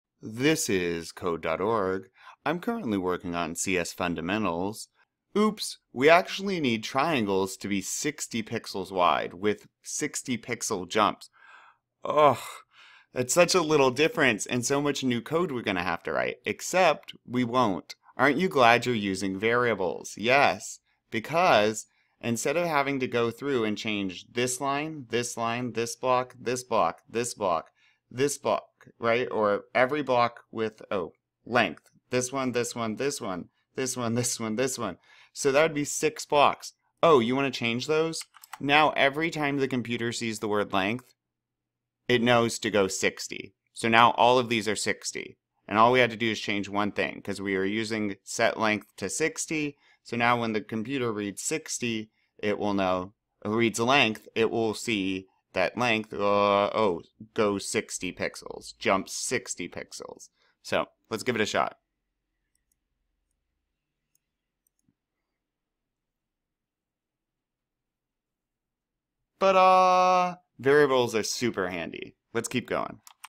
Code.org Variables with Artist Answers Explained | Course F Lesson 8.8 | Express Lesson 19.9
Variables with Artist is lesson 8 in Course F and lesson 19 in the Express Course. It is part of Code.org's C.S. Fundamentals Course. This is an excellent way to learn to code. Each part challenges students to accomplish a new task while learning computer science concepts. This particular activity,  C.S. Fundamentals is often used in elementary. In this video, I walk through step-by-step how to get the right answer for this puzzle. Full lesson playlists below.